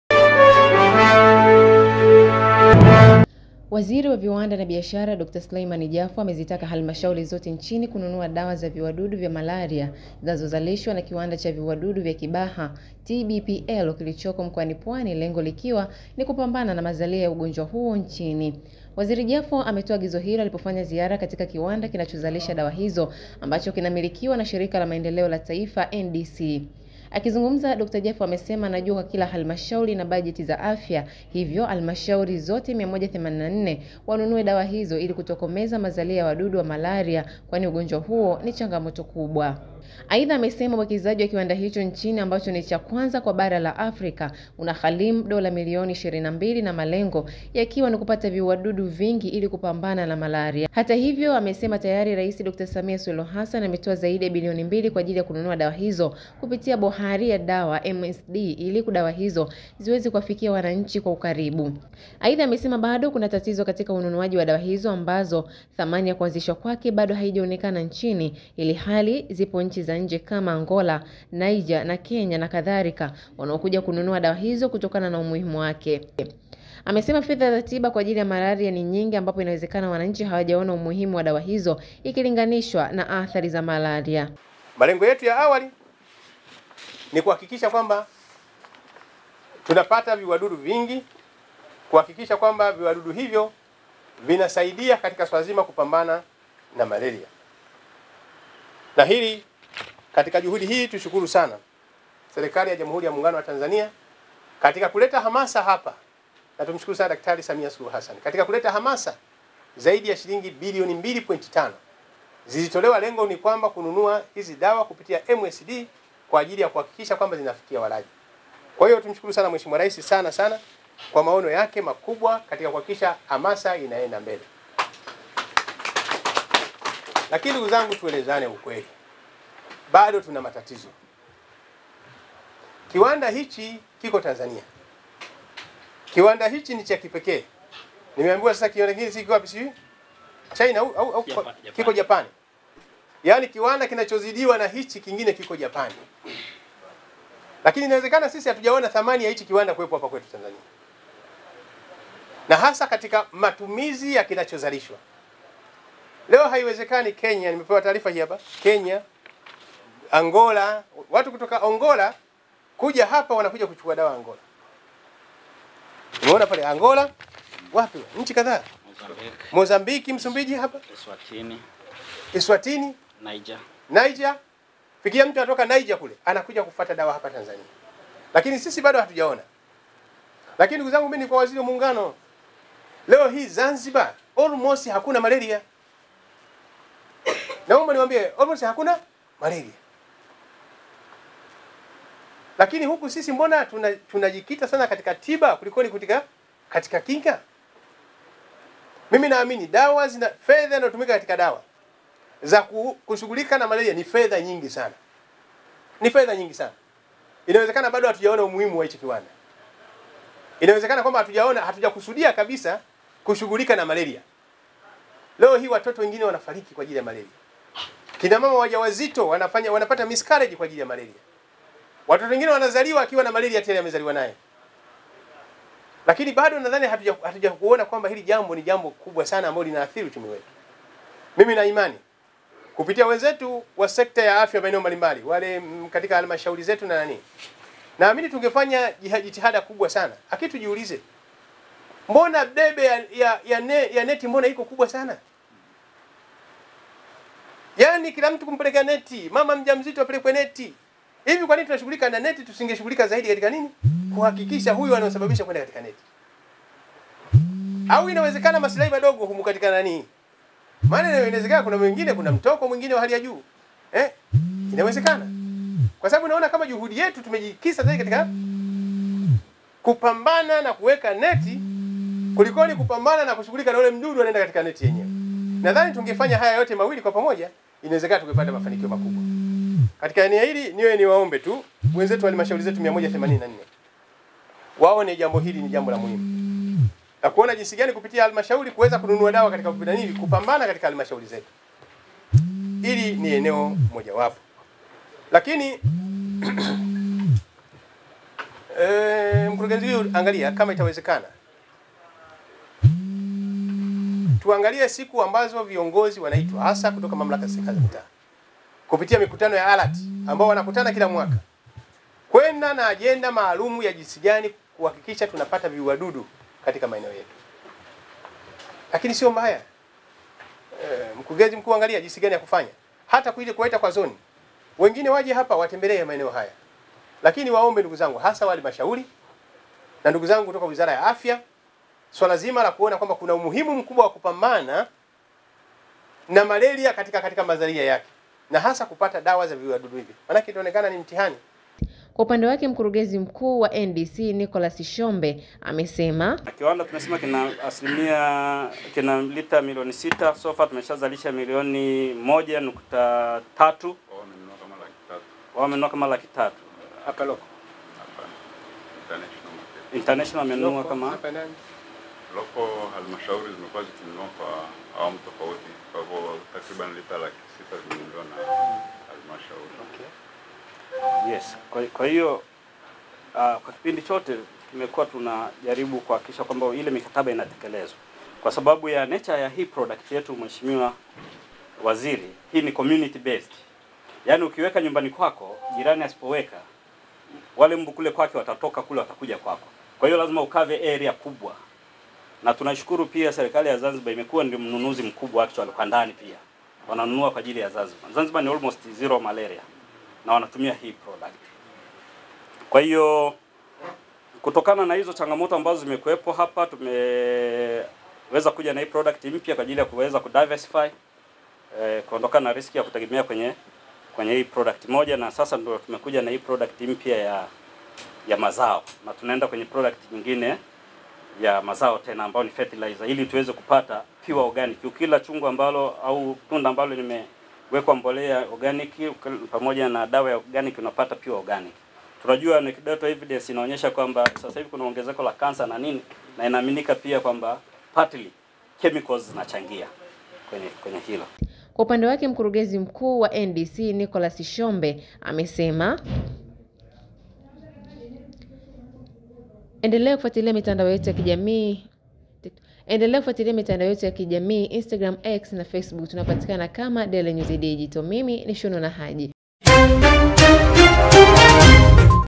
ALICHOKISEMA JAFO ALIVYOTINGA KIWANDA CHA VIUADUDU
WAZIRI wa Viwanda na Biashara Dk Selemani Jaffo amezitaka halmashauri zote nchini kununua dawa za viuadudu  vya malaria, zinazozalishwa na  kiwanda Cha viuadudu cha Kibaha  (TBPL) kilichoko mkoani Pwani lengo likiwa ni kupambana na mazalia ya ugonjwa huo nchini.